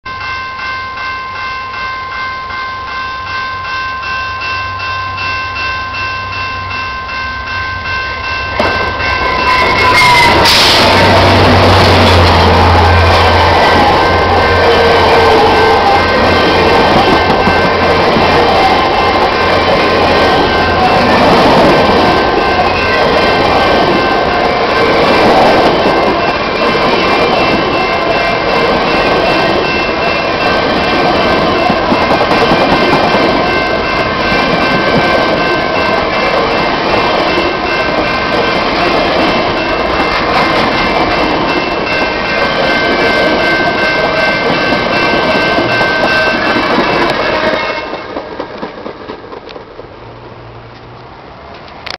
VIA 2 with 6402 and 6452
VIA's Toronto bound Canadian passes through Thornhill 45-60 minutes late with two rebuilt F40s on the head end.